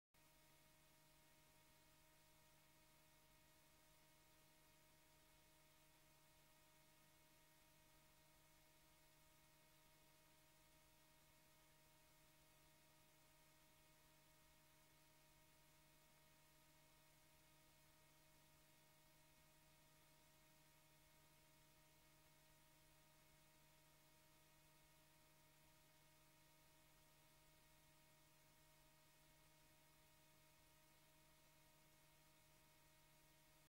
750,000 ste amsterdammer born (Weeknummer 30-04) 🔴
Description (more): Images of the newborn baby, Jan N, which developed the 750,000 ste inhabitant of Amsterdam.
-Nurse puts baby in arms of mother to bed 19 ";
-cu father with son 26 ";